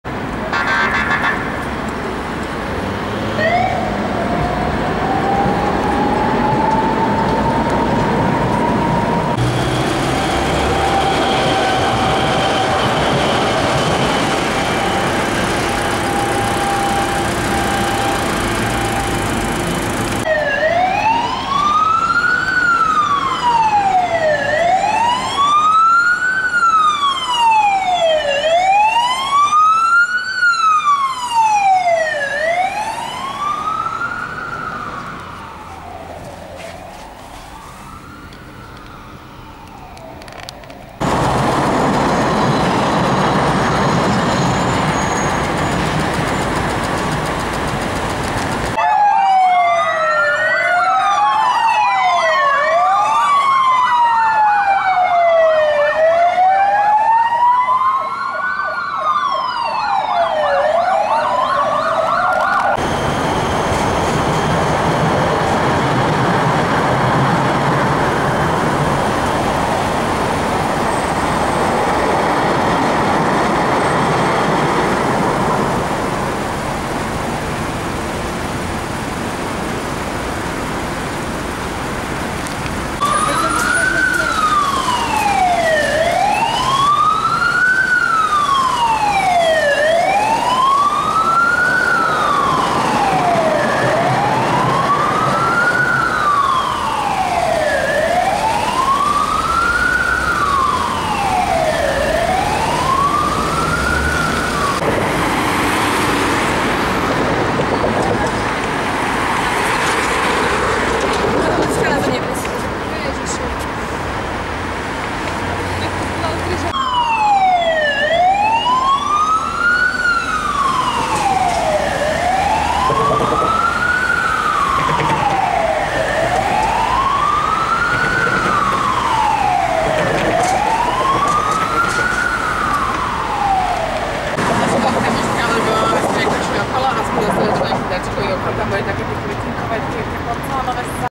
Prague fire dept. response & backing in compilation #4 | Praha HZS sestřih 4 [CZ | 2 + 6.2014]
Prague fire department response and backing in compilation featuring 4 responses and 6 clips of rigs returning. Note that there's a police cruiser responding at 1:01 as well which I didn't noticed.
The first clip was taken in February, all the others in June of 2014.

Prague, Czech Republic, 2014.